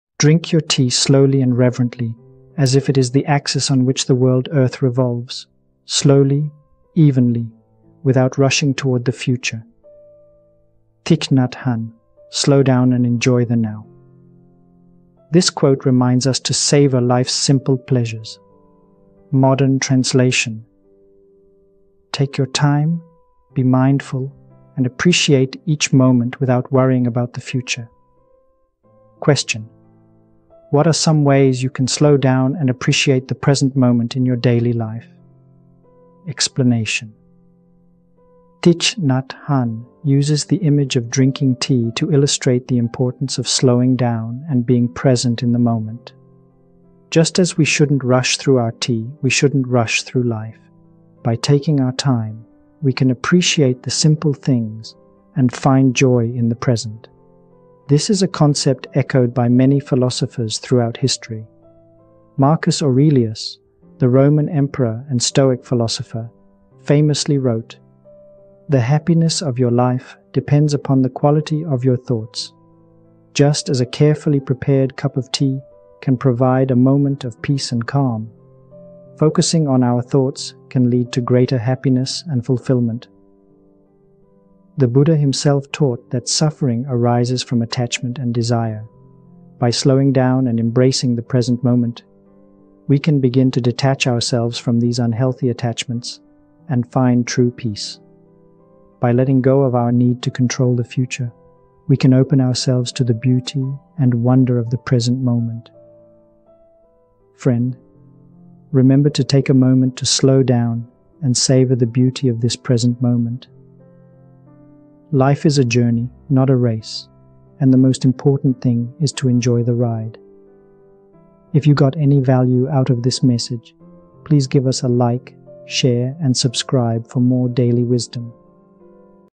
Take your time and appreciate each moment in your life.
Quote Thich Nhat Hanh "Drink your tea slowly and reverently, as if it is the axis on which the world earth revolves  - slowly, evenly, without rushing toward the future.” —Thich Nhat Hanh quotes